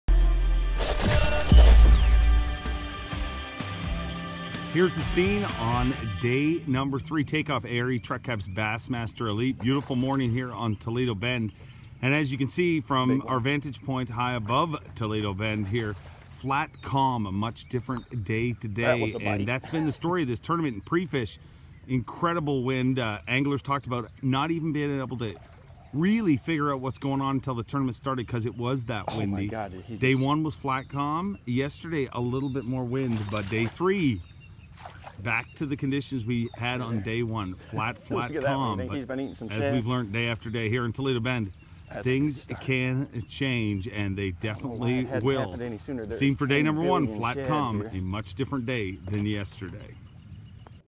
KVD's first catch - day 3 on Toledo Bend
Kevin VanDam starts out Saturday’s fishing with a bang — 4-pounder — on Toledo Bend. Bass master Elite Series 2016. Full bass fishing tournament updates, standings, pictures videos and more